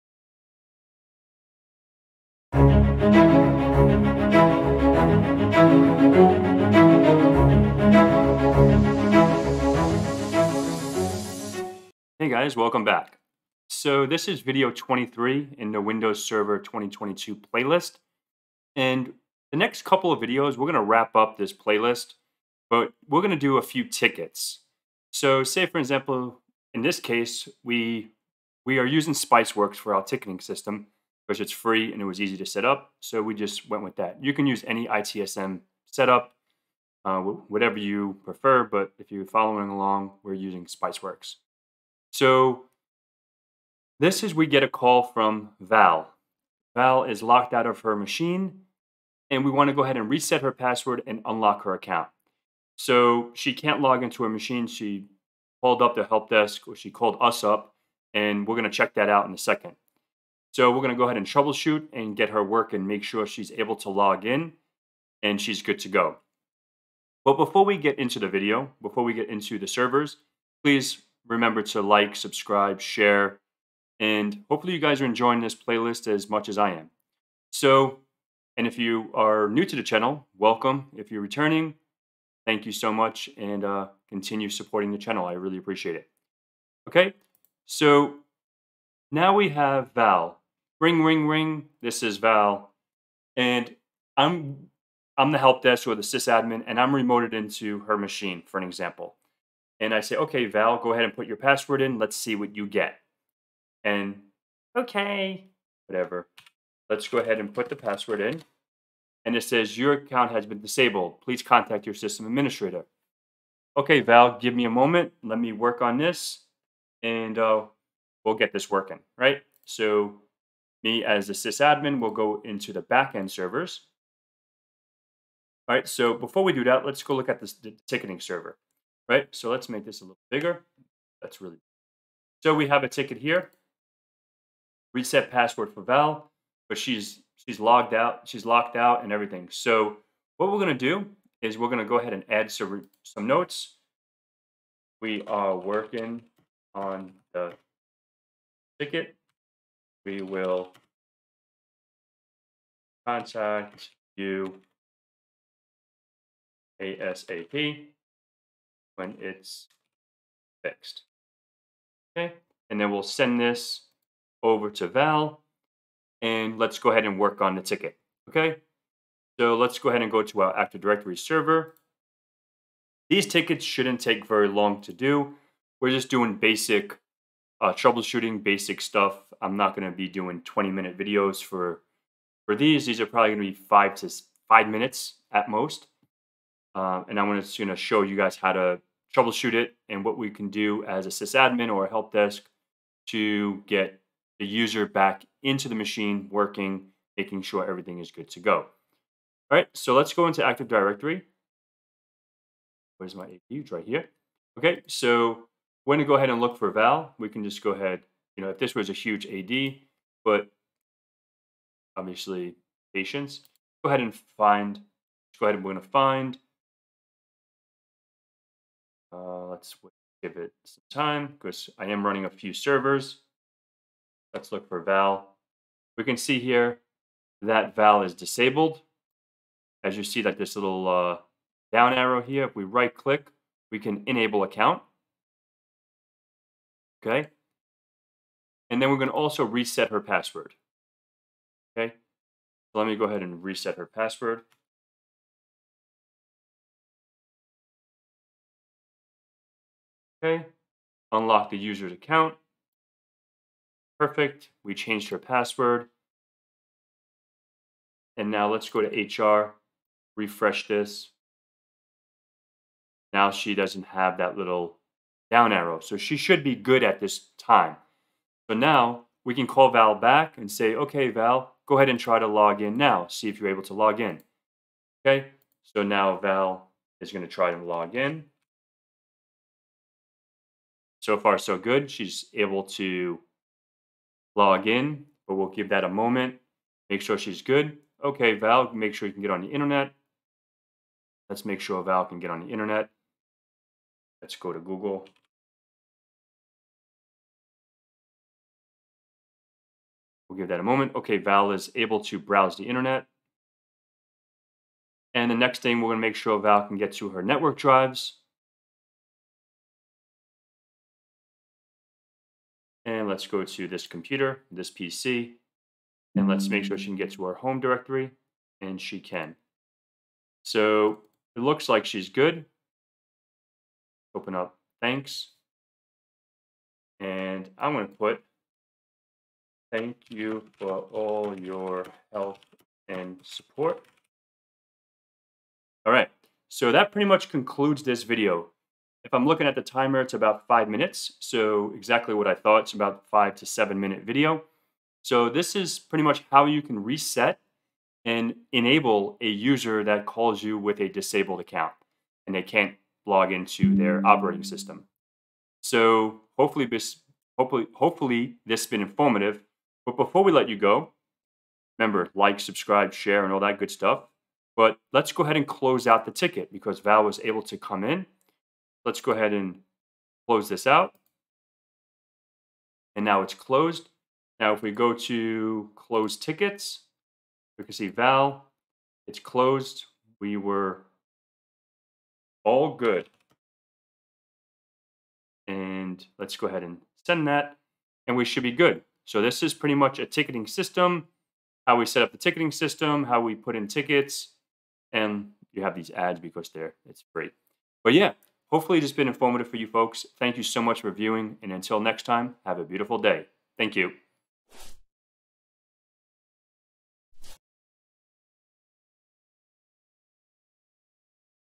How to unlock and reset password windows Server 2022 - Video 23 with InfoSec Pat
Check out the Windows Server 2022 Playlist for the complete training.

Add Infosec Pat on Instagram!